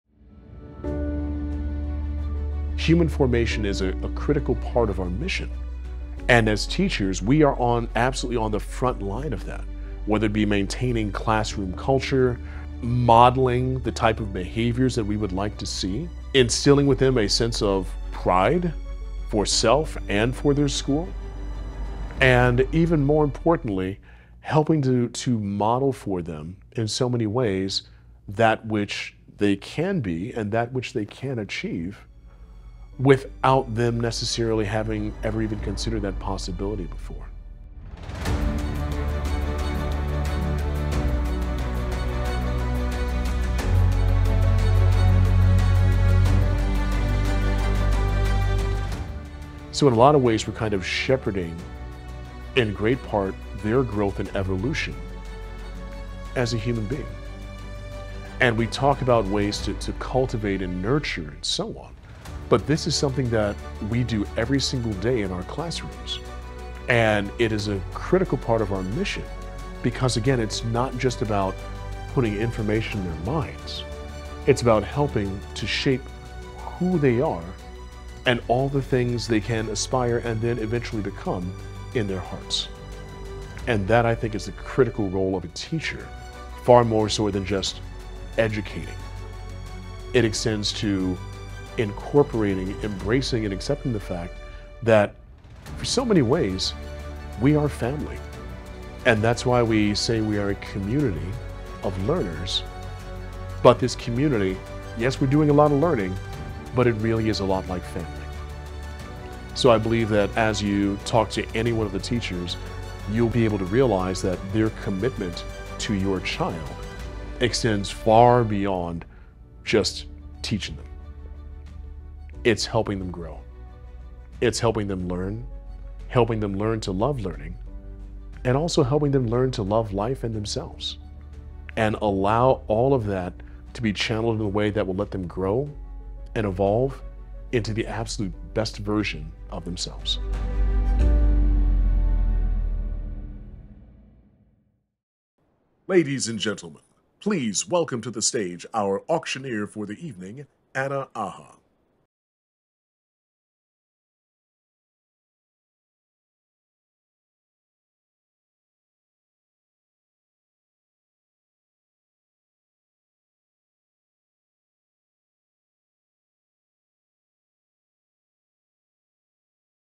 What it Means to be a Teacher with Clifton Kiser
Great Hearts Master Teacher, Clifton Kiser, shares what it means to be a teacher at Great Hearts.